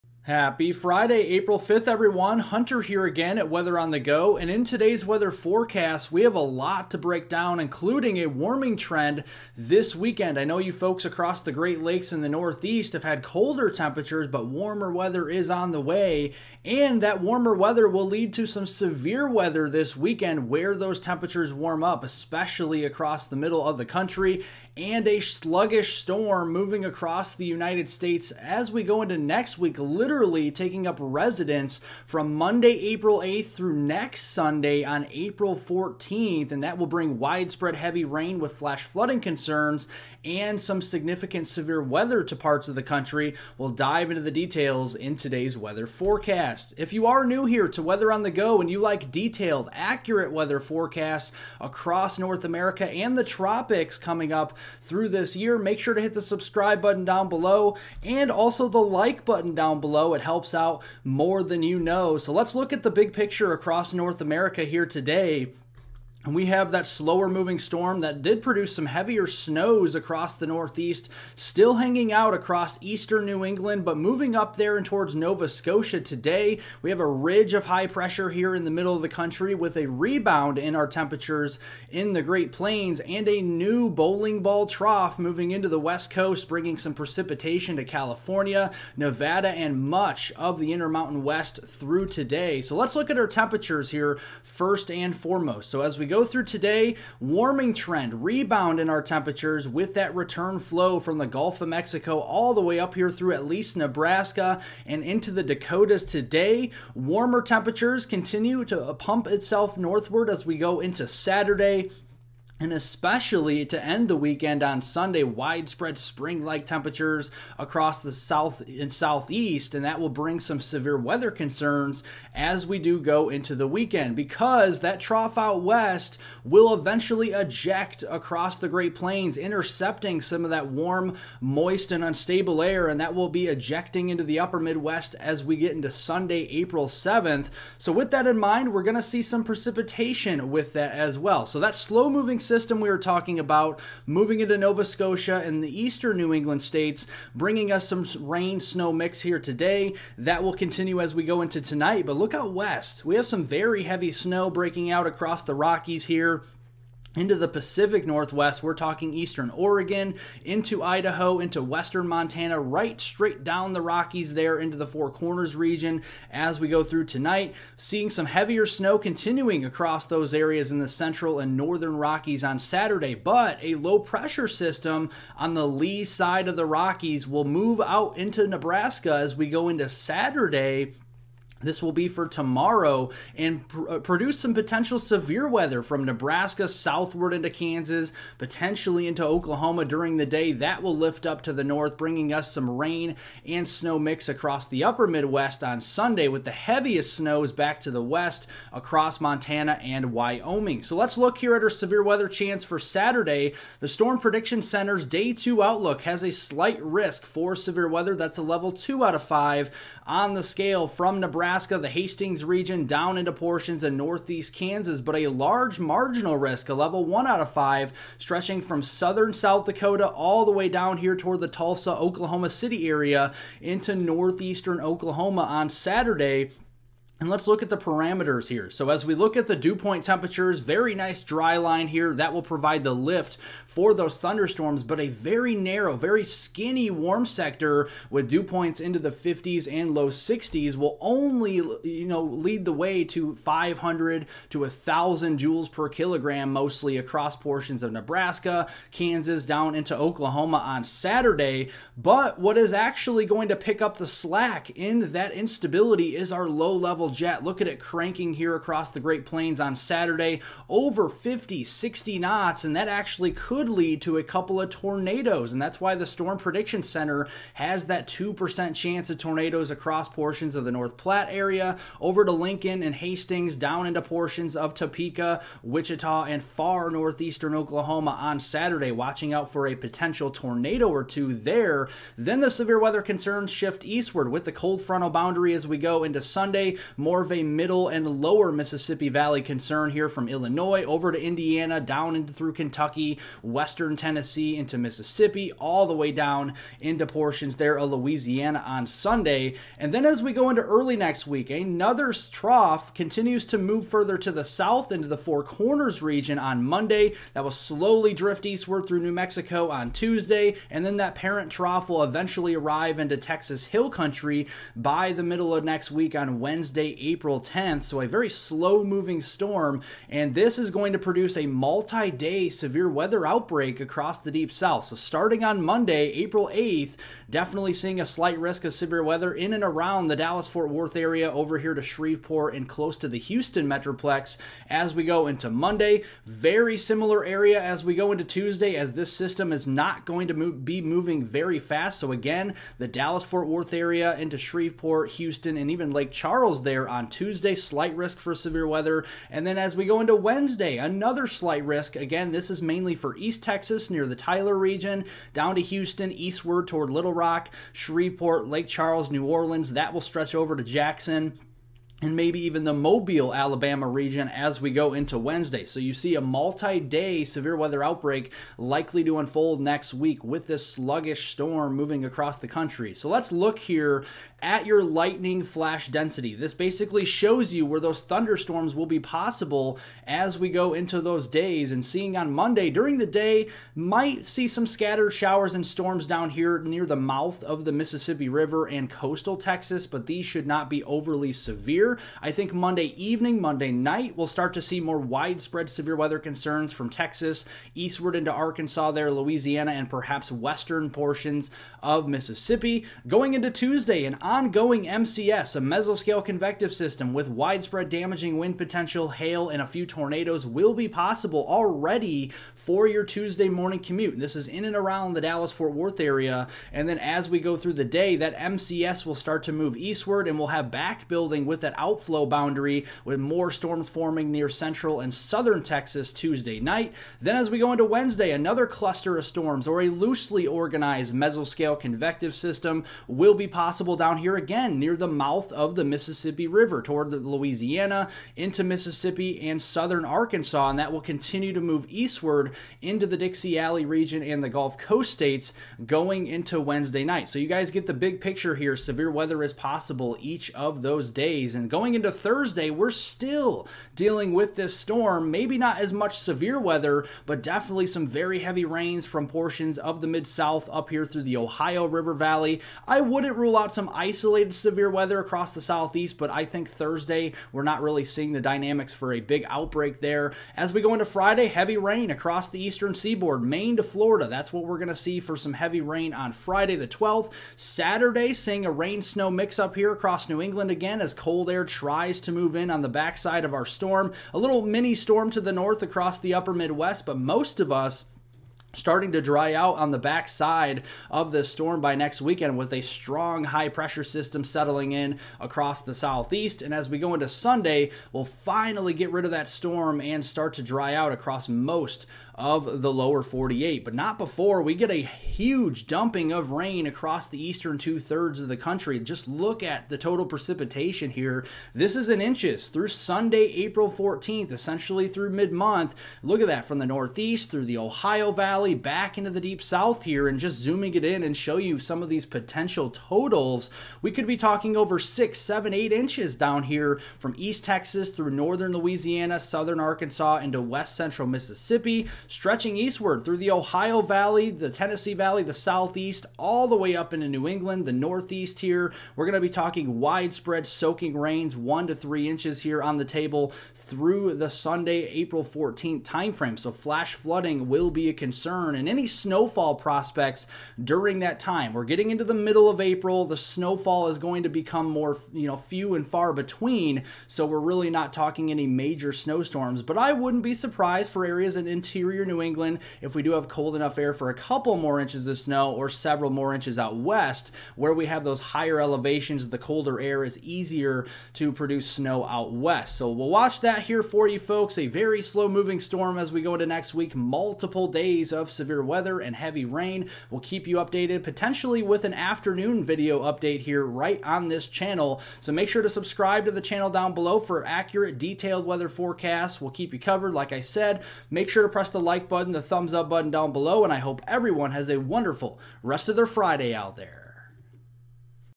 BIG Storms Are Coming!!!
A multi-day severe weather outbreak is likely to unfold next week across parts of the United States! Damaging winds, large hail, and tornadoes will be possible. Heavy rainfall and flash flooding will also be possible.

VIDEO CHAPTERS:
0:00 Overview
1:01 Warming Trend
2:03 Weekend Storm
4:59 Multi-Day Outbreak
7:31 Heavy Rainfall
9:32 Outro